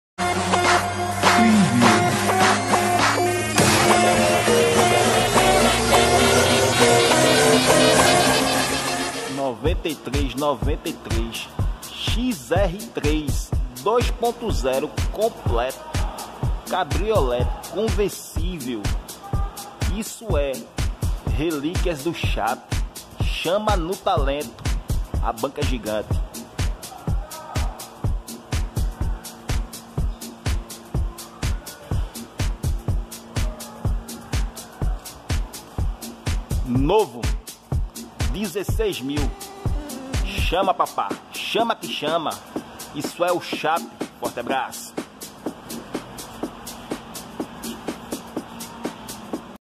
Carros do Chape Ford Escort XR3 1993
🏁😎😱 Relíquias do Chape!

Ford Escort XR3 2.0 Completo

1993/1993 Zero R$ 16.000 Negociável

Documentação 2020 em dias Recibo em Branco.

Veiculo em perfeito estado de conservação.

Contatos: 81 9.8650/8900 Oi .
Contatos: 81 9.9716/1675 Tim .

Olinda/Recife/Pernambuco/Brasil.